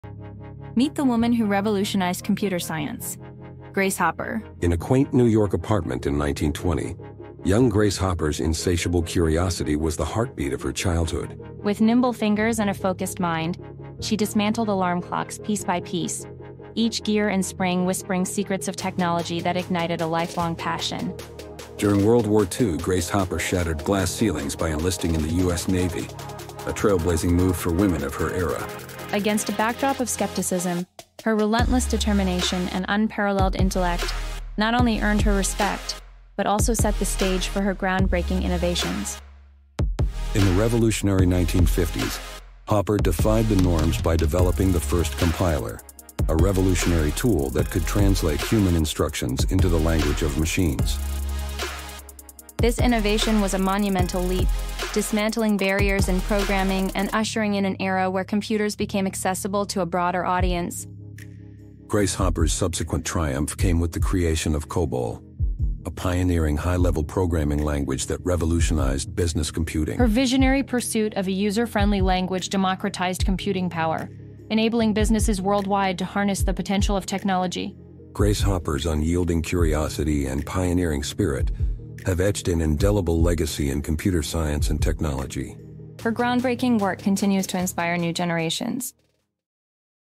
Meet the Woman Who Revolutionized Computer Science
Explore the remarkable journey of Grace Hopper, whose groundbreaking work in the U.S. Navy and creation of the first compiler and COBOL revolutionized the world of computer science. Discover how her relentless curiosity and pioneering spirit continue to inspire generations of innovators.